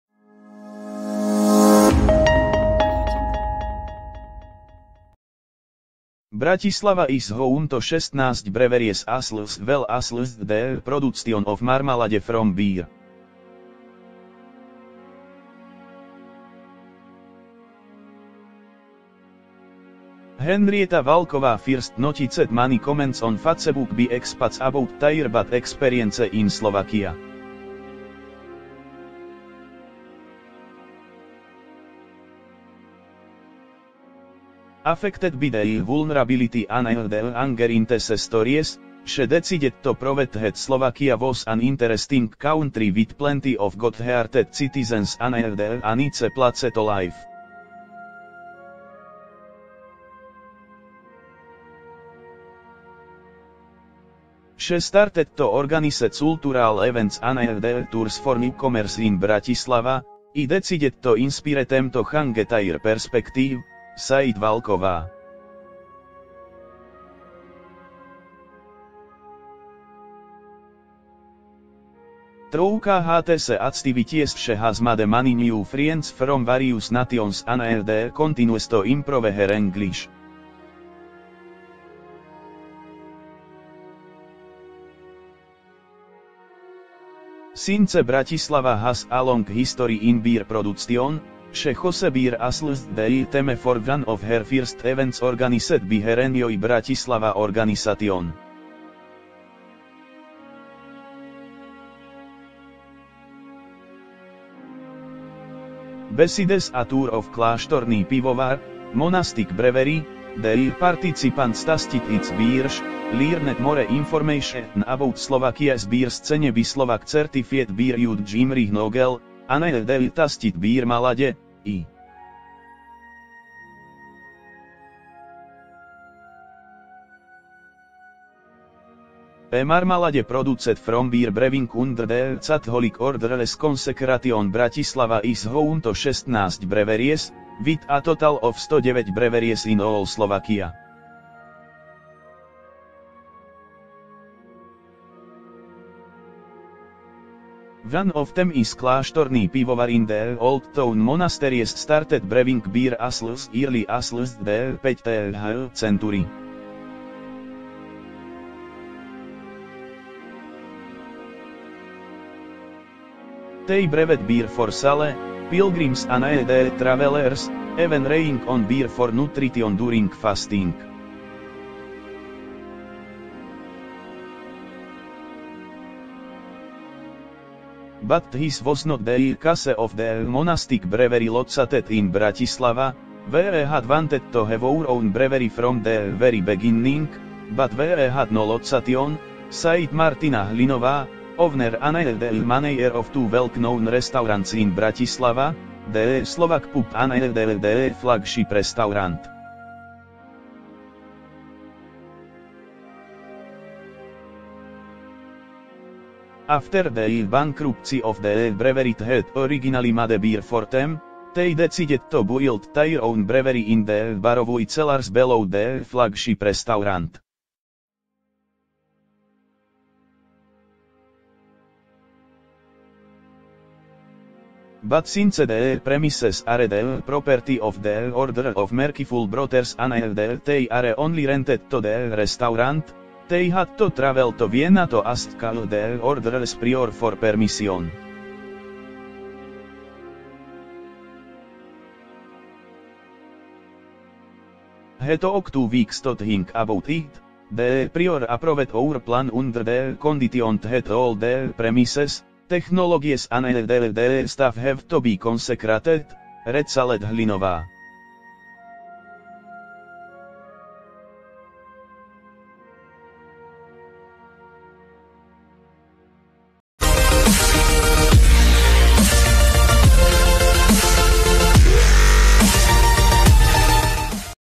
Getting to know Bratislava better through beer
Bratislava is home to 16 breweries as well as the production of marmalade from beer.
Henrieta Valková first noticed many comments on Facebook by expats about their bad experience in Slovakia. Affected by the vulnerability and anger in these stories, she decided to prove that Slovakia was an interesting country with plenty of good-hearted citizens and a nice place to live. She started to organise cultural events and tours for newcomers in Bratislava.

“I decided to inspire them to change their perspective,” said Valková. Through these activities she has made many new friends from various nations and continues to improve her English.

Since Bratislava has a long history in beer production, she chose beer as the theme for one of her first events organised by her Enjoy Bratislava organisation. Besides a tour of Kláštorný Pivovar (Monastic Brewery) the participants tasted its beers, learned more information about Slovakia’s beer scene by Slovak certified beer judge Imrich Nógell, and tasted beermalade, i.e. marmalade produced from beer.

Brewing under the Catholic order’s consecration
Bratislava is home to 16 breweries, with a total of 109 breweries in all Slovakia. One of them is Kláštorný Pivovar in the Old Town.

Monasteries started brewing beer as early as the 5th century. They brewed beer for sale, pilgrims and travellers, even relying on beer for nutrition during fasting. But this was not the case of the Monastic Brewery located in Bratislava.
“We had wanted to have our own brewery from the very beginning, but we had no location,” said Martina Hlinová, owner and manager of two well known restaurants in Bratislava – the Slovak Pub and the Flagship Restaurant.

After the bankruptcy of the brewery that originally made beer for them, they decided to build their own brewery in the Baroque cellars below the Flagship Restaurant.

But since the premises are the property of the Order of Merciful Brothers and they are only rented to the restaurant, they had to travel to Vienna to ask the order’s prior for permission. He took two weeks to think about it.

“The prior approved our plan under the condition that all the premises, technologies and the staff have to be consecrated,” recalled Hlinová.